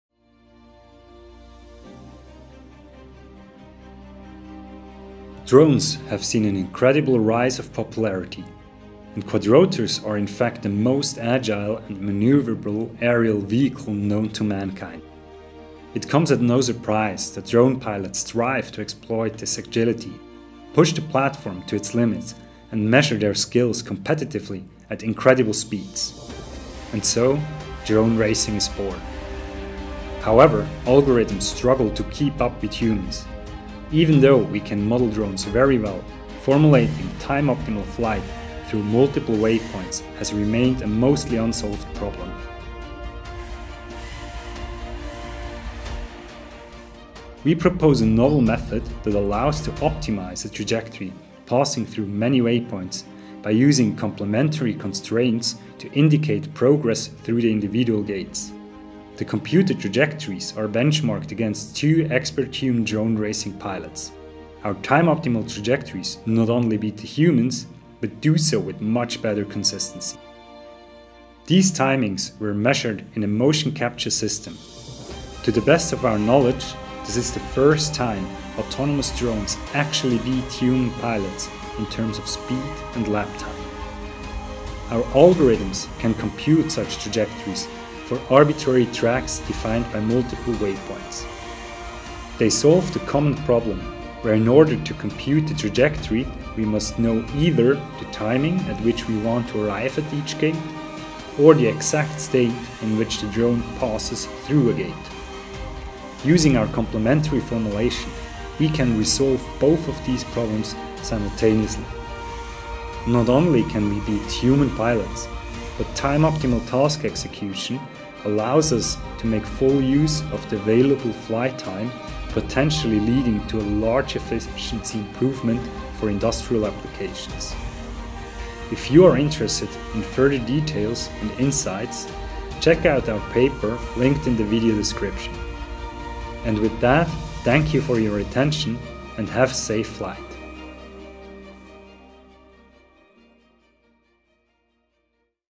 Time-Optimal Planning for Quadrotor Waypoint Flight (Science Robotics 2021)
Quadrotors are among the most agile flying robots. However, planning time-optimal trajectories at the actuation limit through multiple waypoints remains an open problem. This is crucial for applications such as inspection, delivery, search and rescue, and drone racing. Early works used polynomial trajectory formulations, which do not exploit the full actuator potential because of their inherent smoothness. Recent works resorted to numerical optimization but require waypoints to be allocated as costs or constraints at specific discrete times. However, this time allocation is a priori unknown and renders previous works incapable of producing truly time-optimal trajectories.
To generate truly time-optimal trajectories, we propose a solution to the time allocation problem while exploiting the full quadrotor’s actuator potential. We achieve this by introducing a formulation of progress along the trajectory, which enables the simultaneous optimization of the time allocation and the trajectory itself. We compare our method against related approaches and validate it in real-world flights in one of the world’s largest motion-capture systems, where we outperform human expert drone pilots in a drone-racing task.

Reference:
P. Foehn, A. Romero, D. Scaramuzza
"Time-Optimal Planning for Quadrotor Waypoint Flight"
Science Robotics, July 21, 2021

All the authors are with the Dept. of Informatics, University of Zurich, and Dept. of Neuroinformatics, University of Zurich and ETH Zurich, Switzerland